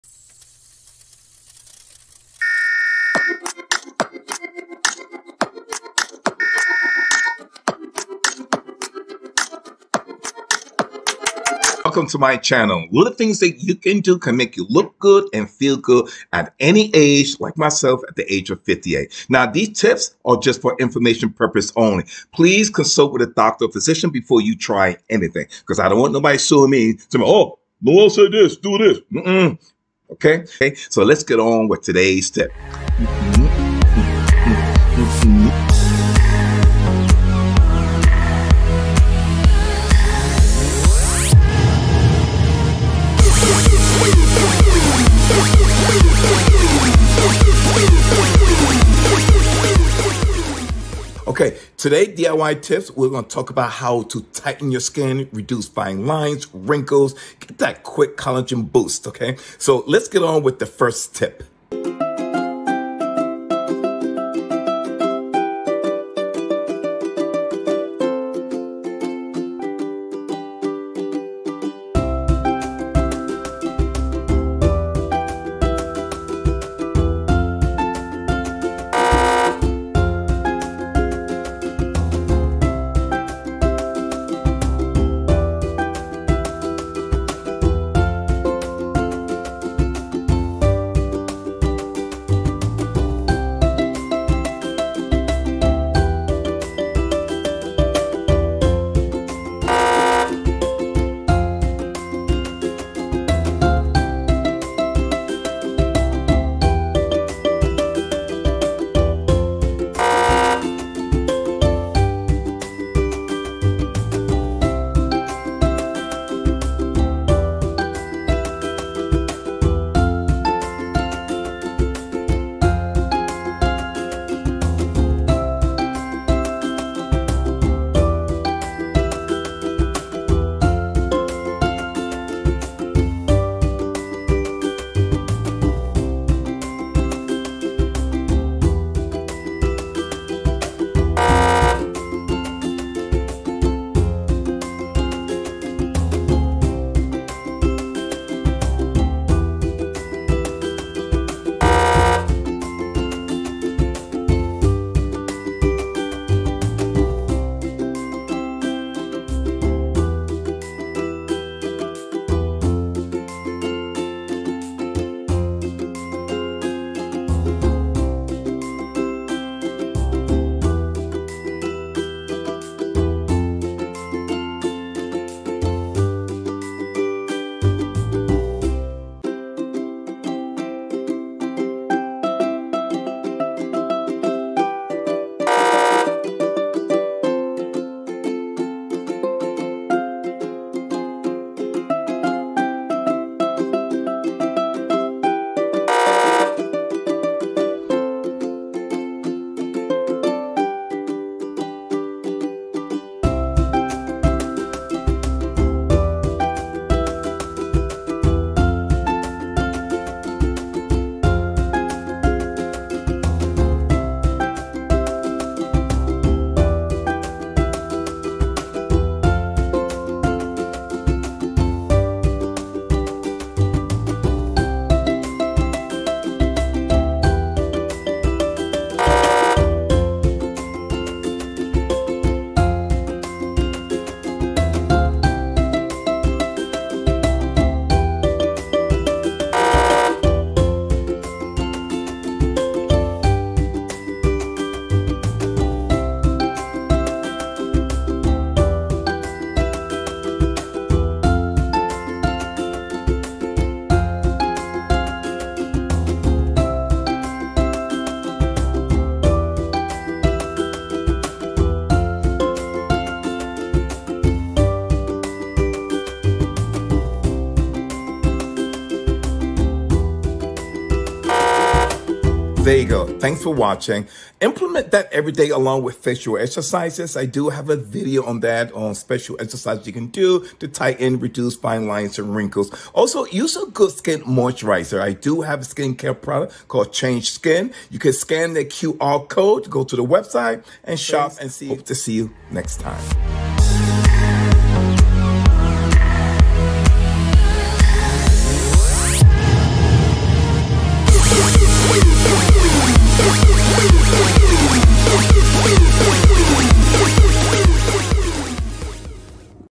How to slim your face - How to stimulate collagen boost. Look younger.
How to slim up your face. How to reduce fine lines and wrinkles. Stimulate collagen boost. Every day simple things you can do can make you feel good, and look good. Do it everyday!

Anti Aging Collagen Boost. Organic Face Mask. How to look more attractive. How to reduce wrinkles.